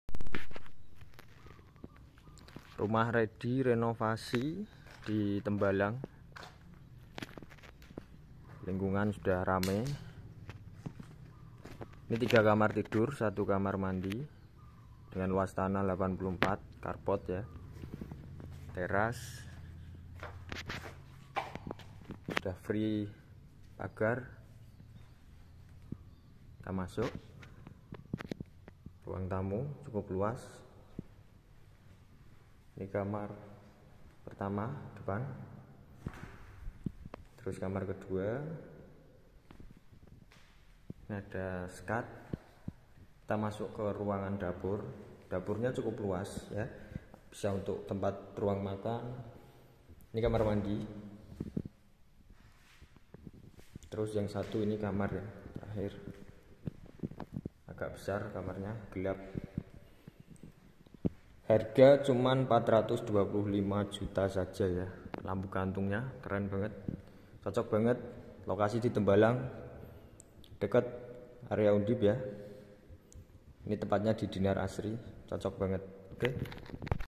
RUMAH MURAH DI TEMBALANG SIAP HUNI 3 KT FULL BANGUNAN
*DIJUAL*
Rumah Dinar Asri, Meteseh, Tembalang

Deskripsi :
Luas Tanah 84 m2 Full bangunan
Dapur
Carport
Listrik 900 Watt
PDAM + Sumur
Jalan bisa simpangan mobil
Lingkungan Asri, Aman, dan Nyaman

Sertifikat, IMB, PBB, Ready

Fasilitas Umum :
- Dekat dengan RS KRMT Wongsonegoro, Primaya Hospital, RSND,
- Dekat dengan Pasar Meteseh
- Dekat dengan UNDIP, POLINES, UNIMUS, POLTEKES
- Dekat dengan Puskesmas, Sekolah, Fasilitas Umum, SPBU, Minimarket

Harga 425JT NEGO
Cash/KPR Dibantu Sampai ACC

*FREE BIAYA BALIK NAMA + KANOPI*